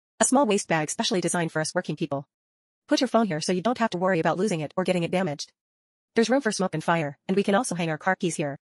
Multi functional waist bag
Title: Multi-functional waist bag: a magical tool for mobile phone protection and pyrotechnic keys in one package
Introduction: This waist bag is specially designed for hard-working people. It perfectly solves the safety problem of mobile phone storage. It can also store smoke and fire, and provide the function of hooking trailer keys. It can not only protect your mobile phone from damage, but also allow you to easily access daily small items anytime and anywhere. It is an indispensable helper in work and life.